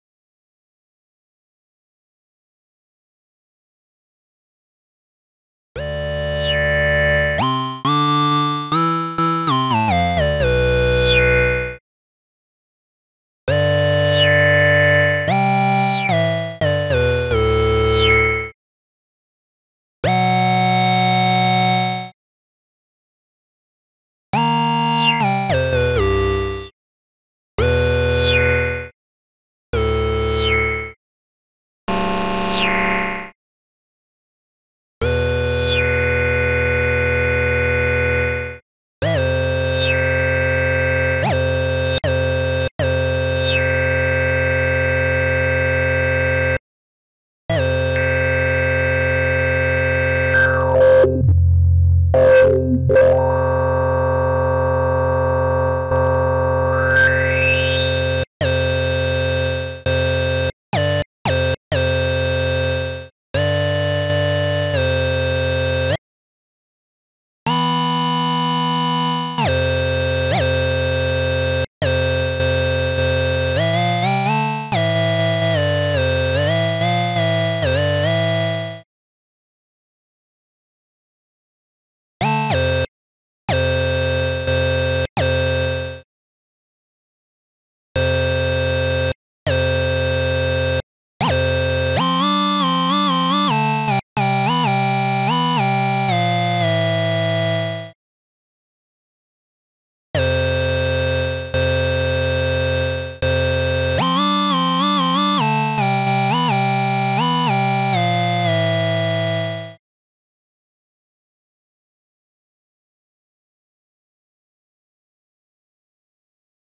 Google Doodle Robert Moog 78th Birthday 22-05-2012
i was really surprised as i saw this in facebook. i hope we got it aswell for my country here.
because i really like moogs - i decided to record it as memory.
we as musician say thank you - Mr. Moog for that stunning Instrument!

Ameyah x